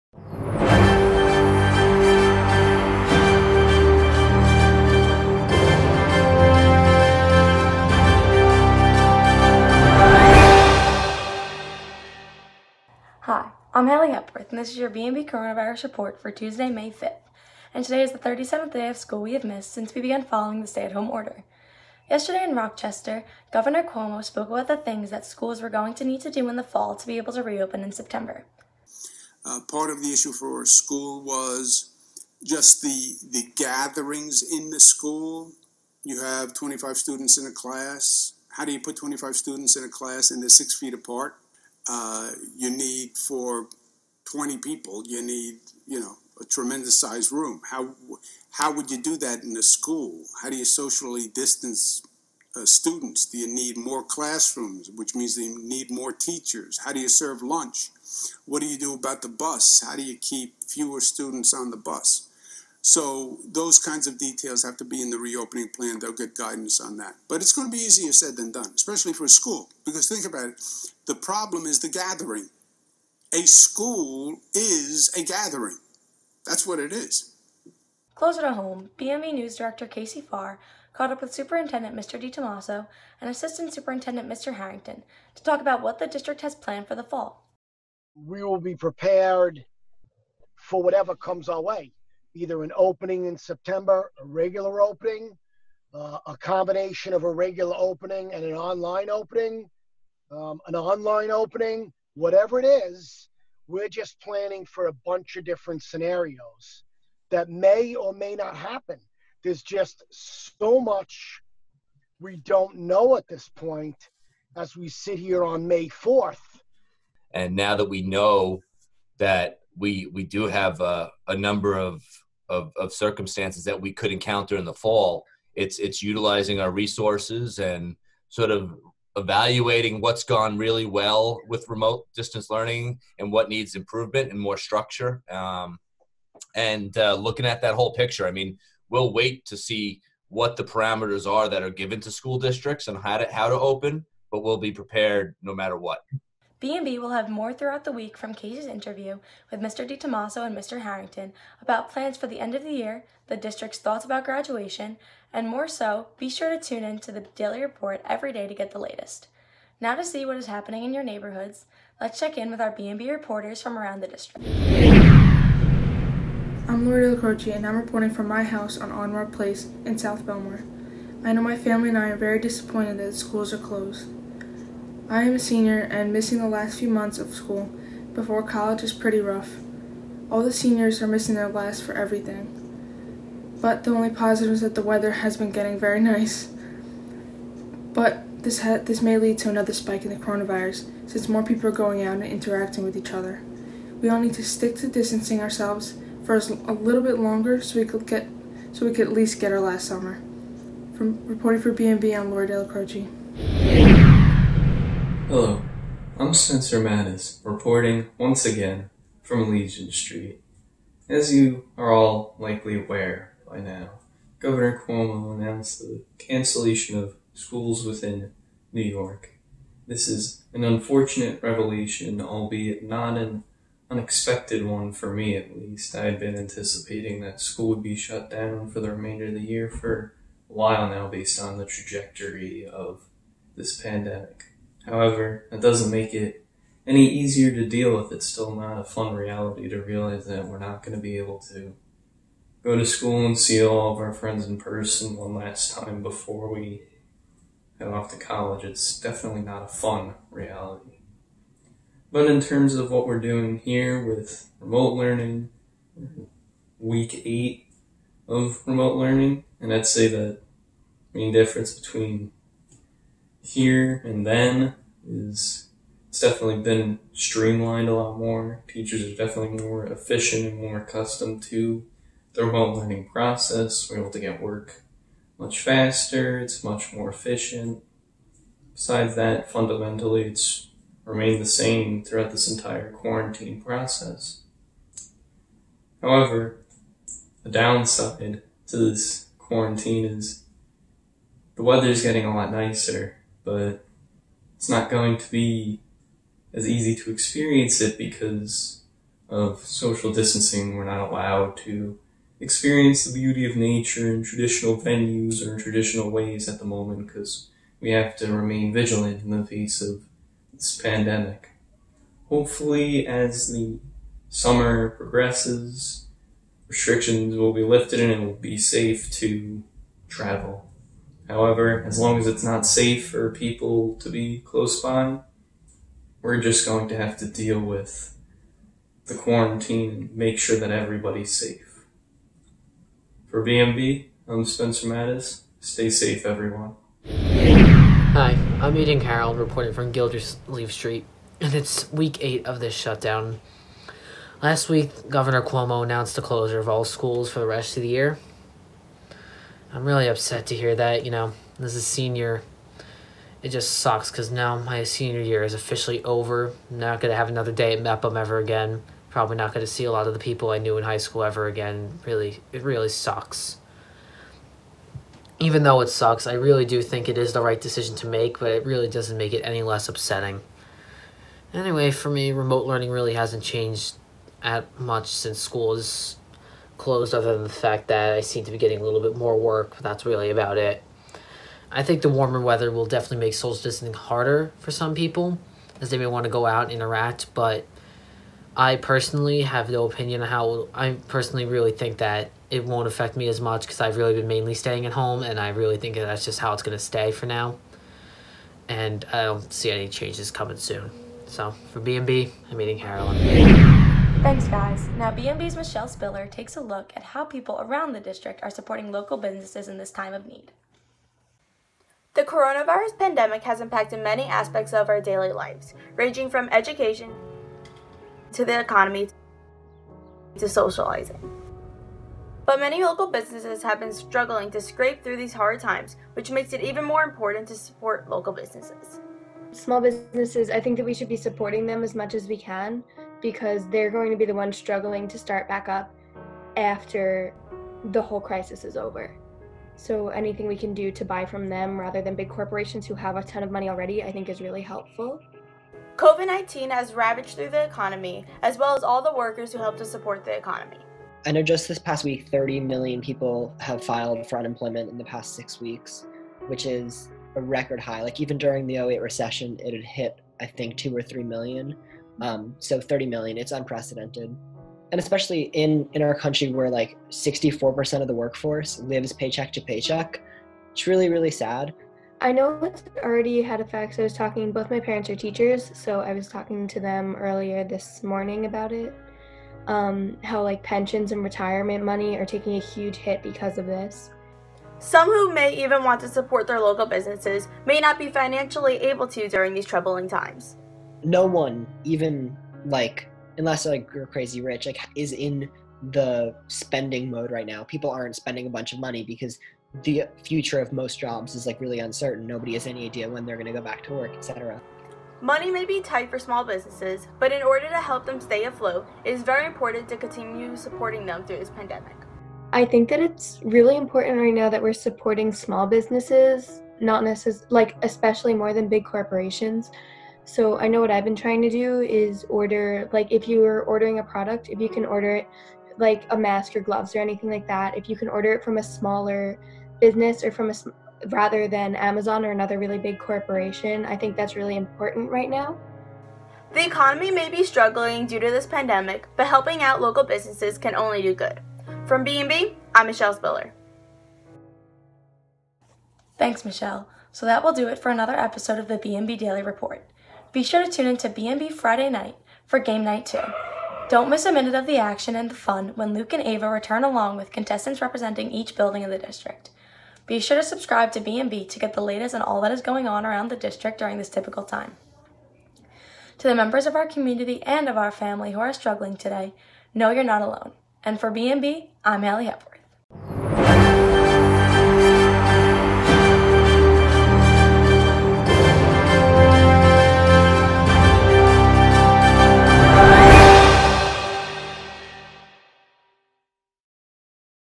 BMB Daily Report for Tuesday May 5th
Haley Hepworth hosts the daily report featuring part of Casey Fahrer's interviews with Superintendent Mr. DeTommaso and Assistant Superintendent Mr. Harrington about the district's plans for next year along with updates from Spencer Mattes, Lorrie DellaCroce and Aidan Carolan along with Michele Spiller's look at ways people in the community can support local businesses.